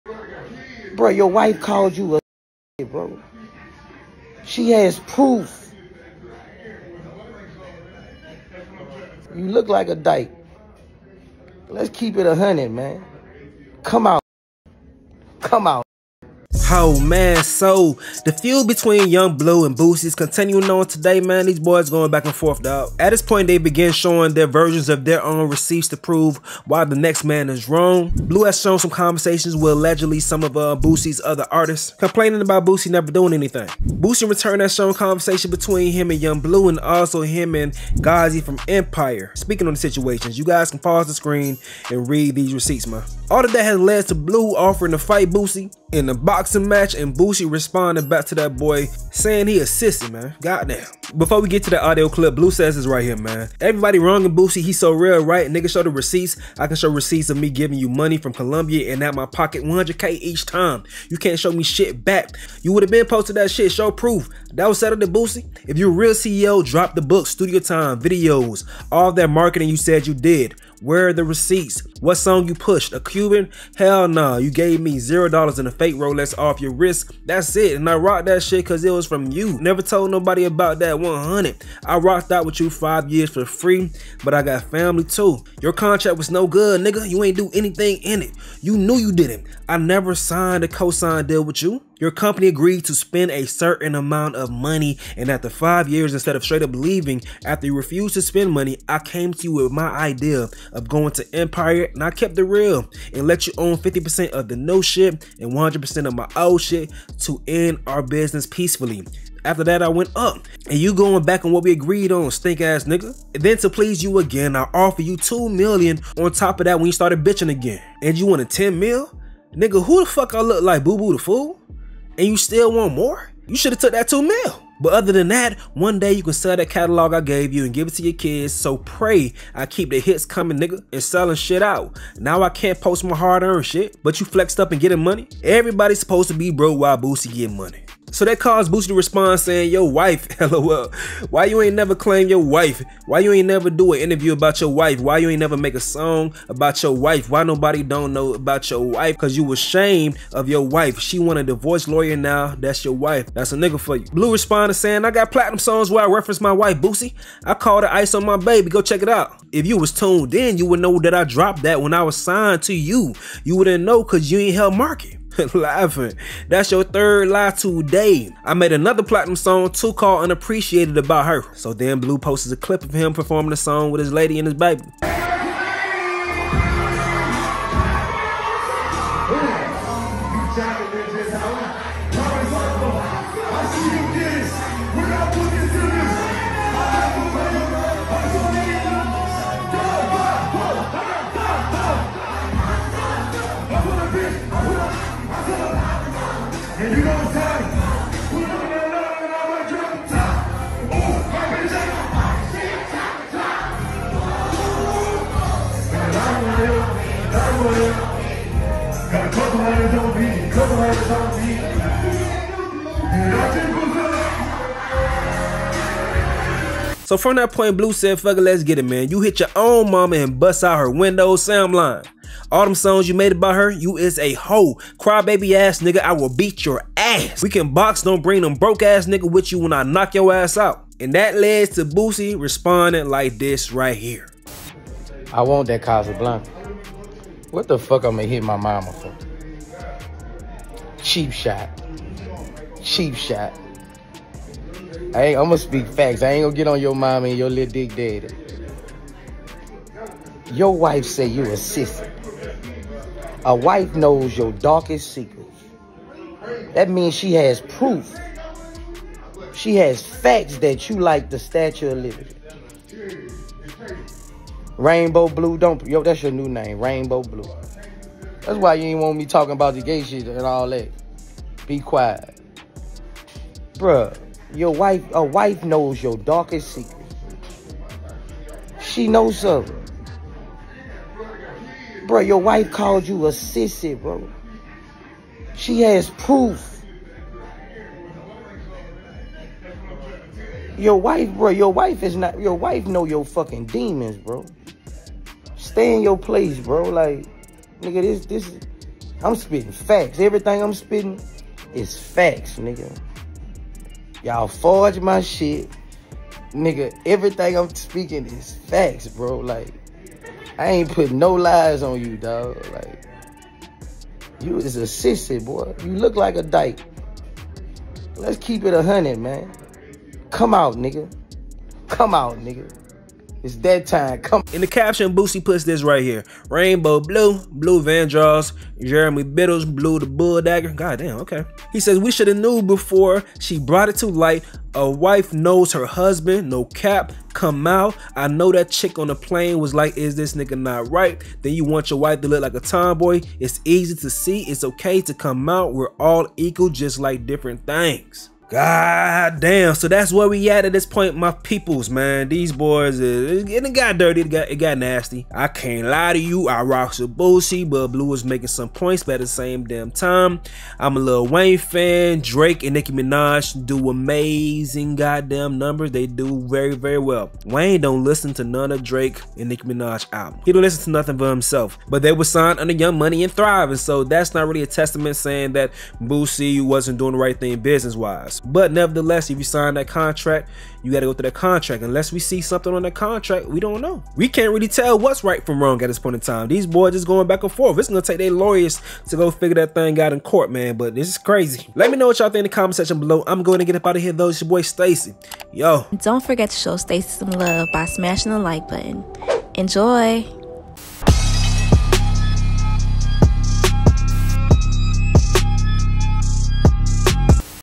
Yung Blue says he'll K.O. Boosie! Boosie Responds "Your Wife called you a S*ssy"
boosie yungbleu Yung Bleu wants to box boosie
Kevin hart races football player